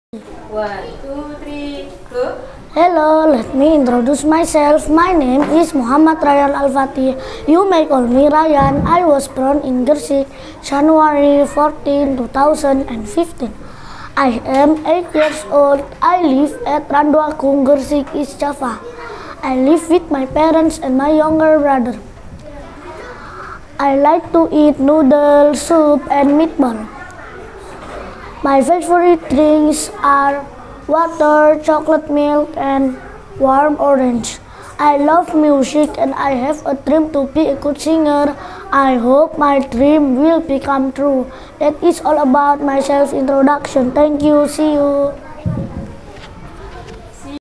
Muhammad Rayyan Al Fatih
Speaking Test "Self Introduction"
1st Grade of ICP
January 2023